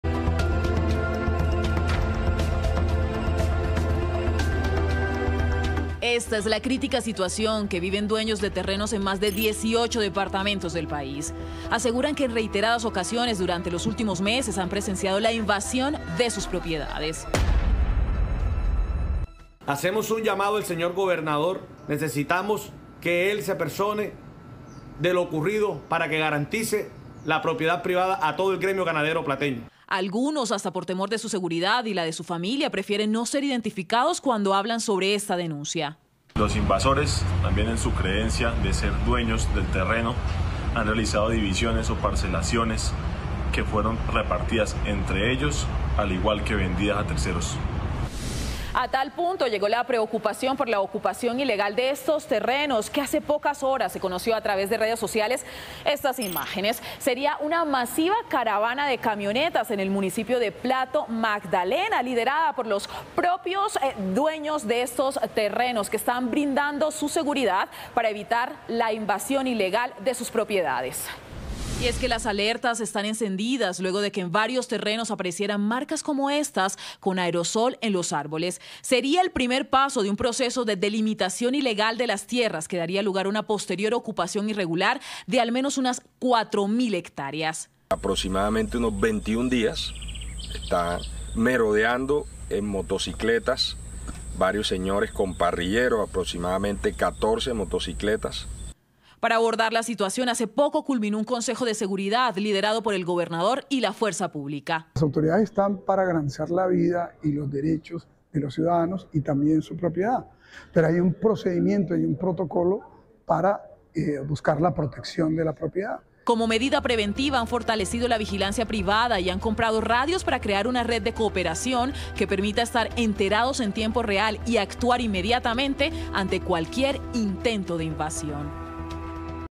Radiografía de la situación en algunas zonas de Colombia por invasión de predios
NoticiasRCN | Ya son más de 18 departamentos del país quienes están sufriendo por la invasión de sus propiedades.